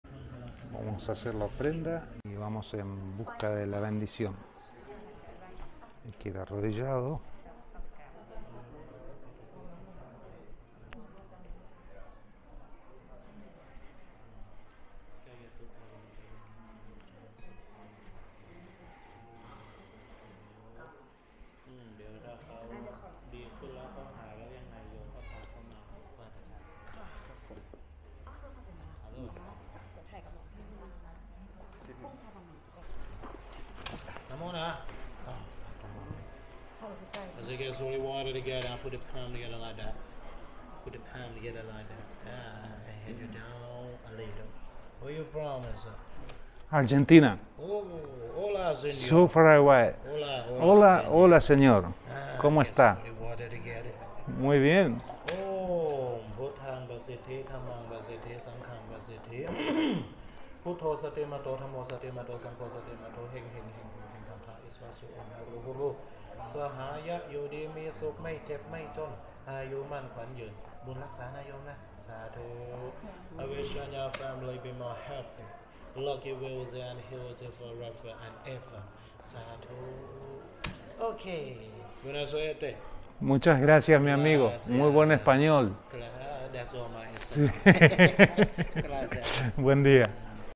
"Bendición Celestial: Experiencia de un Monje en el Templo Doi Suthep"
Aca te muestro solo una parte de lo que va a ser el video sobre la visita al templo Doi Suthep. incluye la bendición de un monje.mirate el trailer para tener una idea del material que se viene!